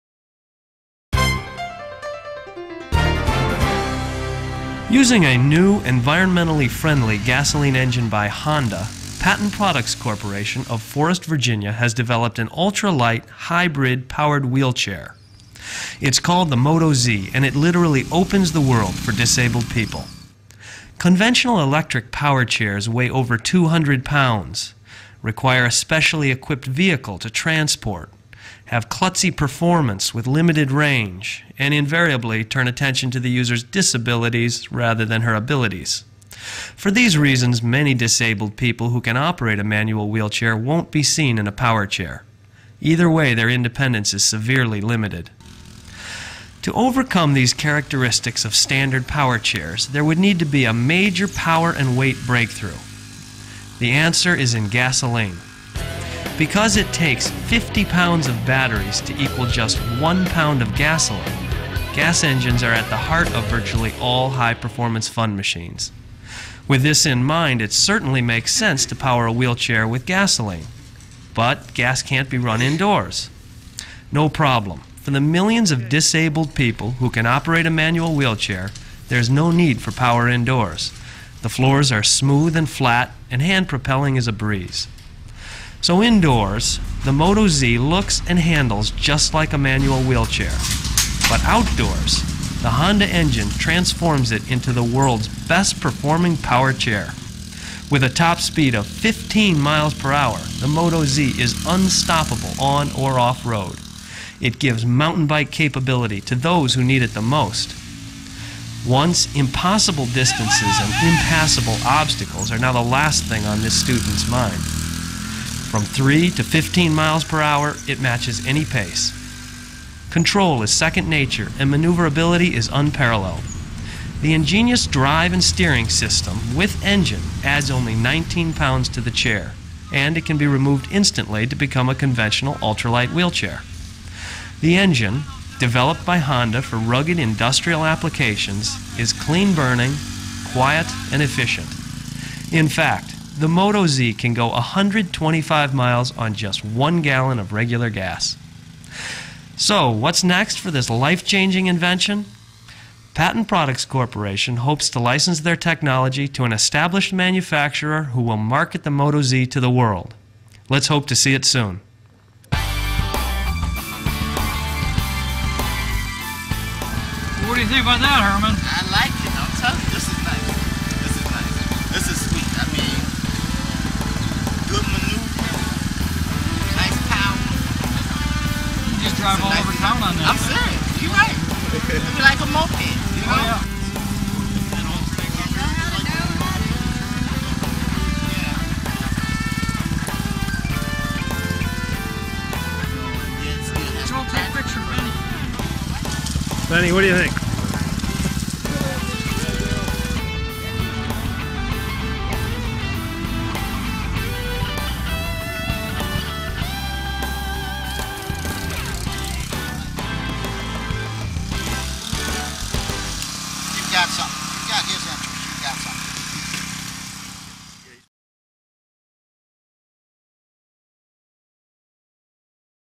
Motochair Propane Powered Wheelchair
The best performing wheelchair of all time.  Since this video, the chair now runs on propane.